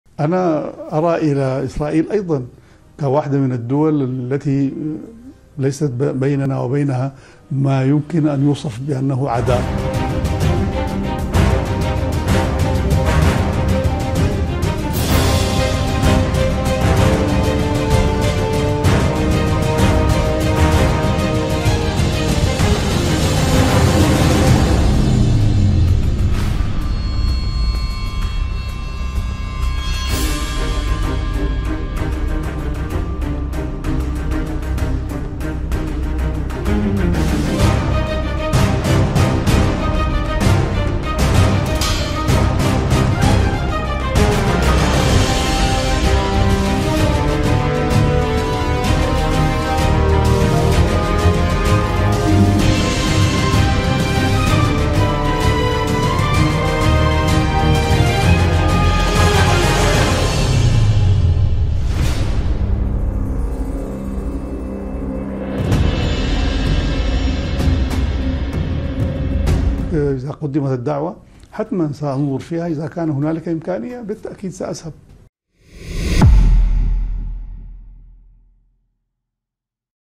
البرهان واسرائيل
لا عداء مع "إسرائيل".. عبد الفتاح البُرهان يُصرُ على جرّ السودان نحو الخيانة ويرحب بزيارة الاحتلال!